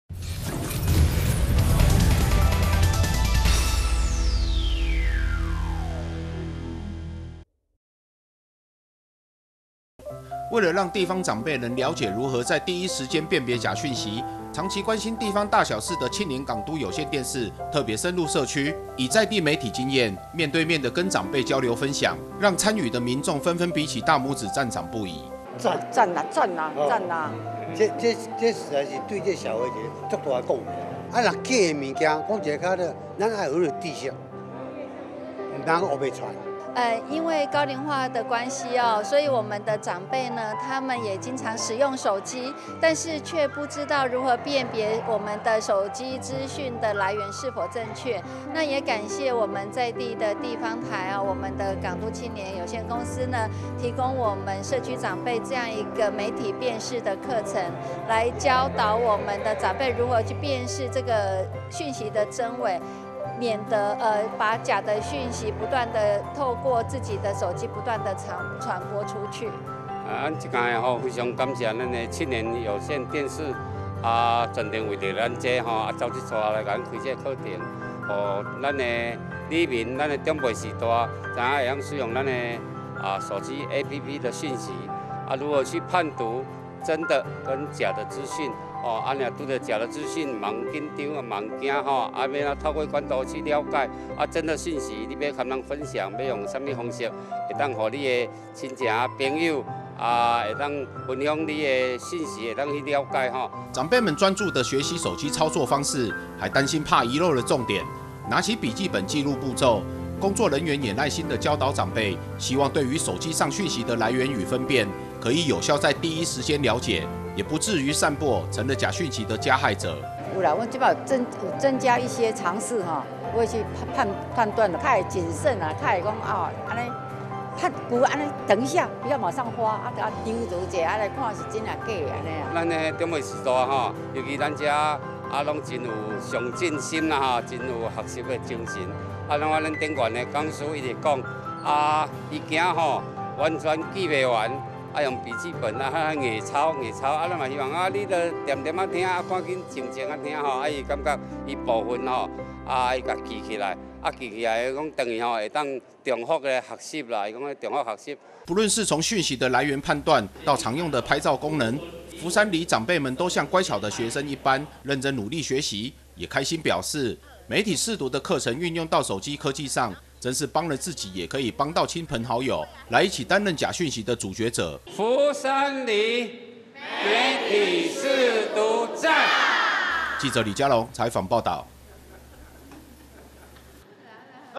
真假 消息|訊息簡訊辨真假媒體識讀長輩樂學習
中嘉集團深入高雄左營區教長輩如何分辨真假訊息

(記者李佳龍/高雄報導)人手一機已是常態，但訊息的真假分辨常是霧裡看花，中嘉集團秉持媒體經驗，深入地方區里，與民眾交流分享，參與學員就如同上了一堂寶貴的課程，對工作人員讚賞不已。